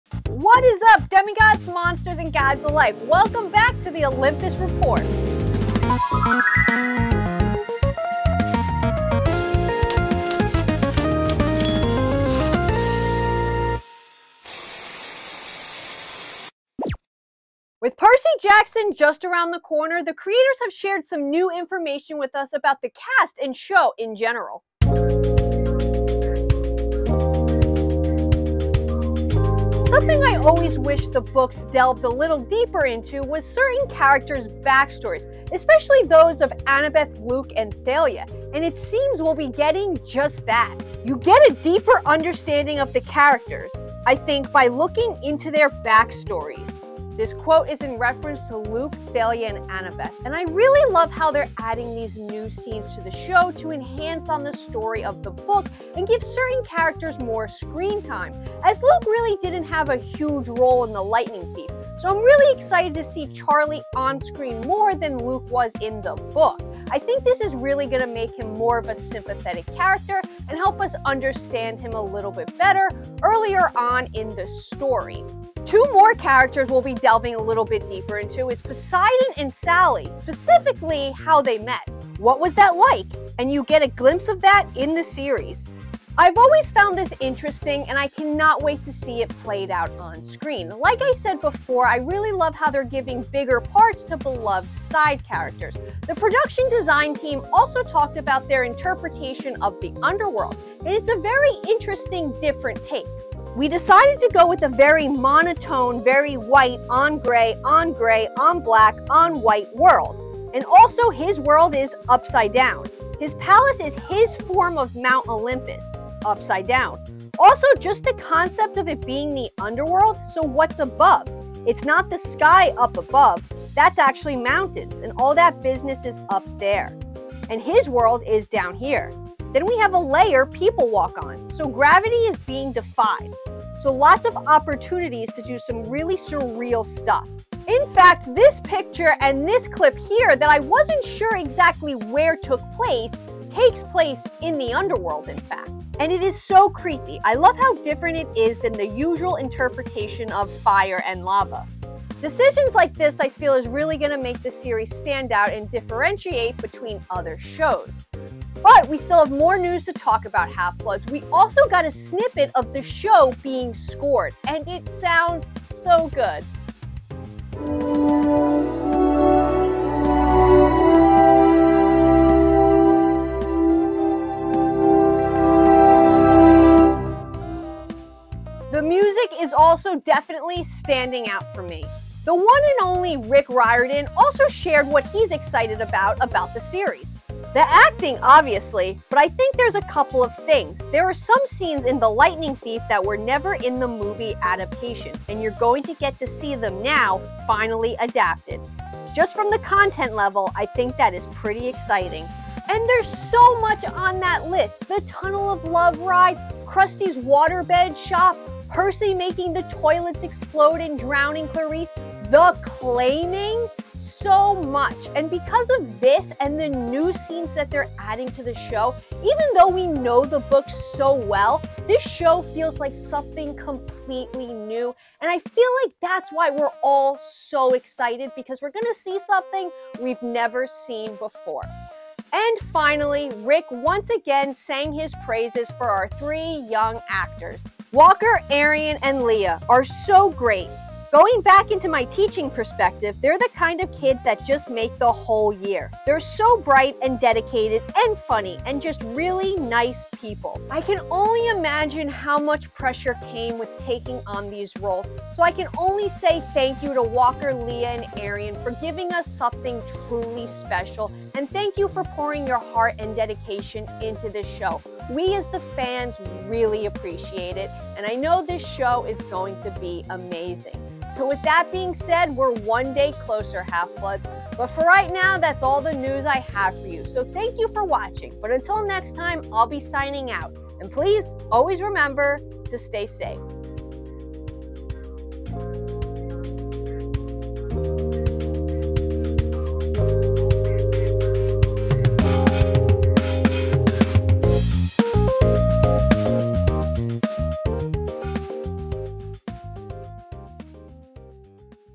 Disney+ Percy Jackson Update / News: Rick Riordan & Director talk New Exclusive Content & Scenes
The Percy series is going to explore new stories we;ve never seen before! Let's take a look at what we can expect!